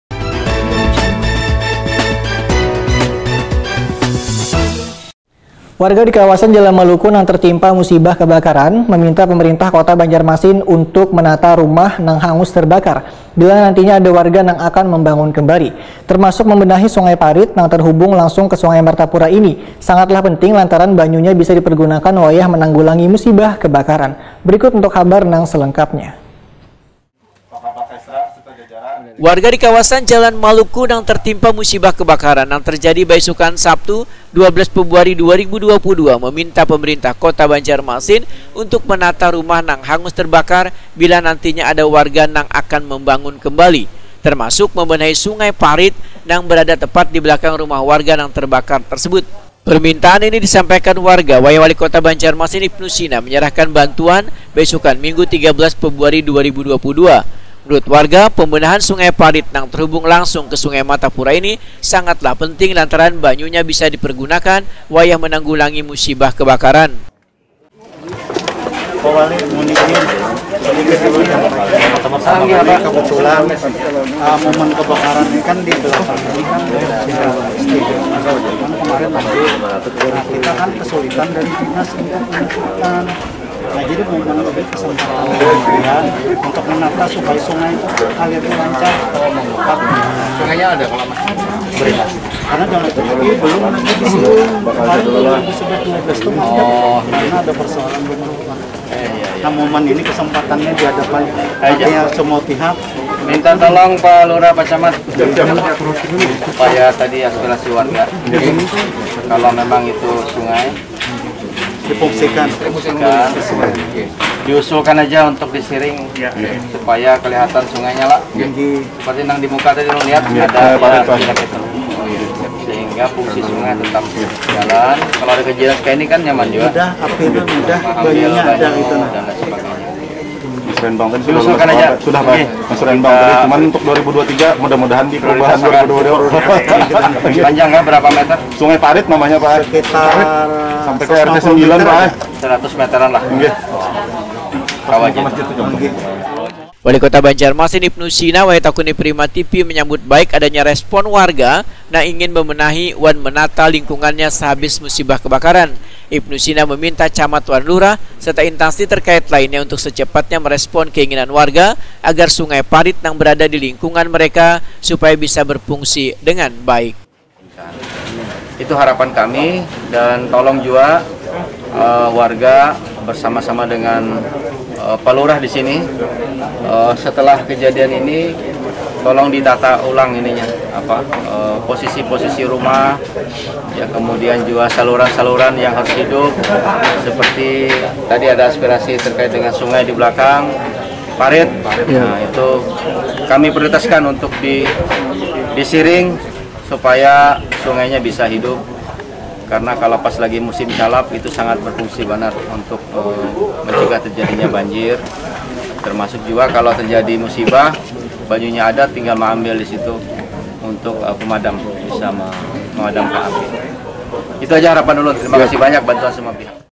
WARGA JALAN MALUKU MEMINTA PEMERINTAH KOTA MENATA RUMAH NANG TERBAKAR WAN MEMBENAHI SUNGAI PARIT
Warga di kawasan  Jalan Maluku nang tertimpa musibah  kebakaran nang terjadi baisukan Sabtu 12 Februari 2022 meminta pemerintah kota Banjarmasin  untuk menata rumah nang hangus terbakar bila nantinya ada warga nang akan membangun kembali//  Termasuk membenahi sungai Parit nang berada tepat di belakang rumah warga nang terbakar tersebut//  Permintaan ini disampaikan warga wayah Walikota Banjarmasin – Ibnu Sina menyerahkan bantuan/ baisukan Minggu 13 Februari 2022//